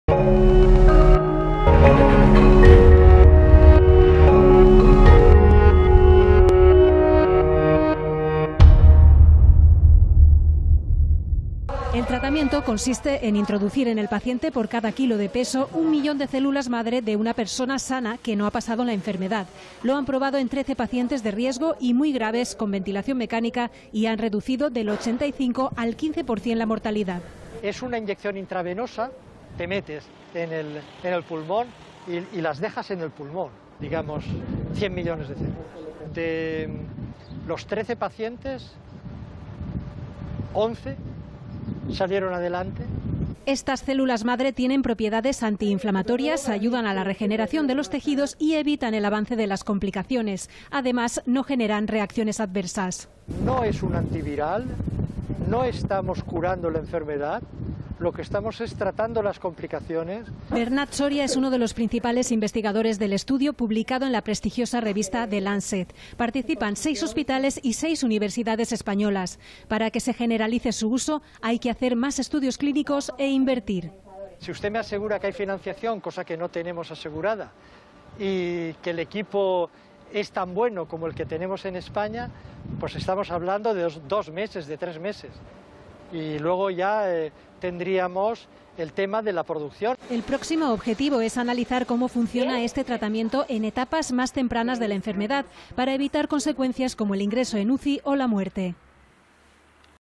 Investigadores españoles desarrollan un TRATAMIENTO celular que REDUCE la mortalidad de COVID-19
Un tratamiento celular desarrollado y probado por investigadores españoles ha demostrado en sus primeros resultados que reduce la mortalidad de los pacientes críticos de coronavirus del 85% al 15%, según han informado este miércoles fuentes de la Universidad Miguel Hernández (UMH) de Elche.

El medicamento celular ha sido probado en trece pacientes intubados en ventilación mecánica, ingresados en UCI, y ha demostrado ser eficiente para la mejoría clínica de casos críticos de COVID-19.